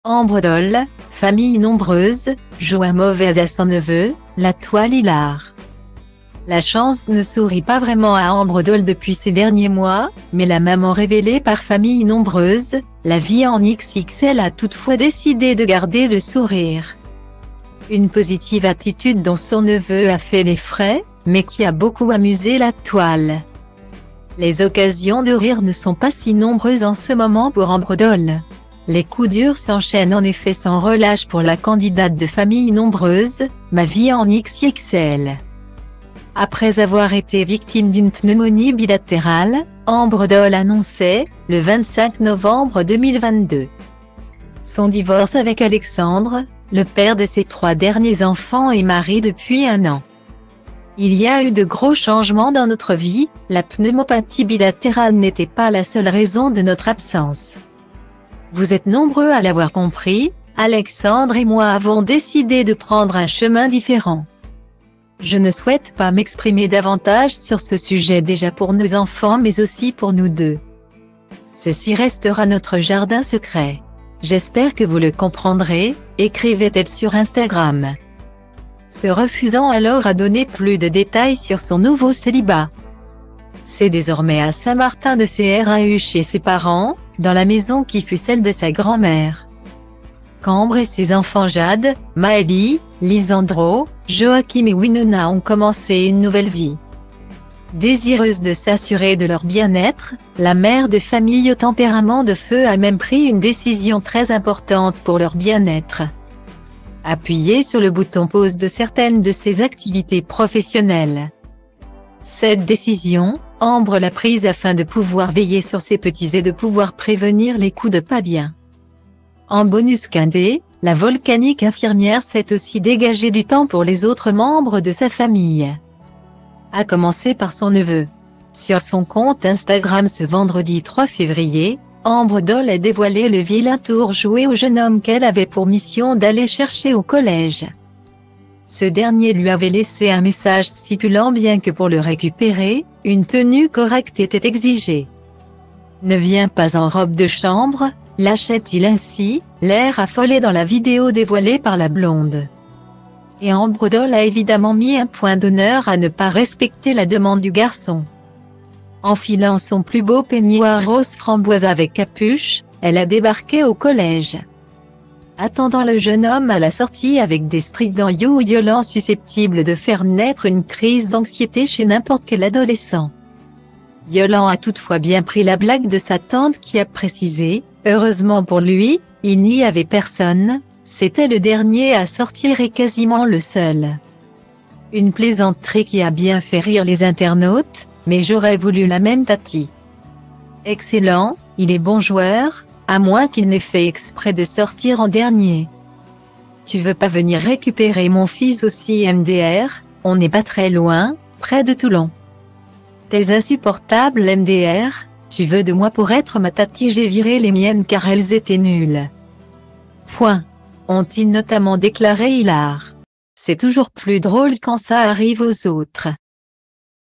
Ambre Dol (Familles nombreuses) joue un mauvais à son neveu, la Toile hilare
La chance ne sourit pas vraiment à Ambre Dol depuis ces derniers mois, mais la maman révélée par Familles nombreuses : la vie en XXL a toutefois décidé de garder le sourire.Une positive attitude dont son neveu a fait les frais, mais qui a beaucoup amusé la Toile. Les occasions de rire ne sont pas si nombreuses en ce moment pour Ambre Dol.